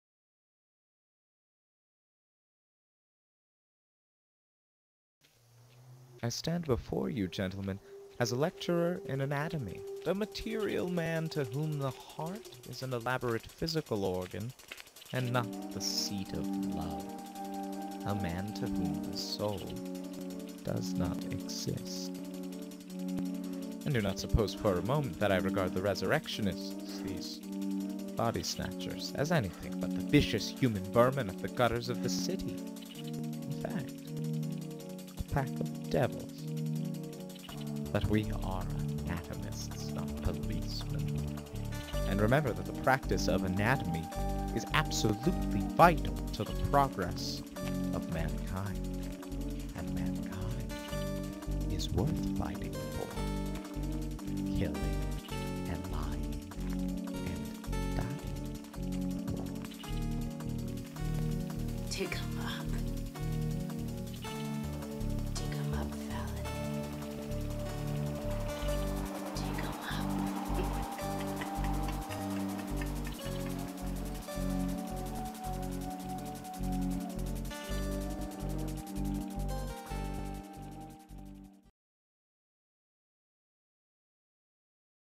The Doctor and The Devils
Trailer for the play "The Doctor and The Devils."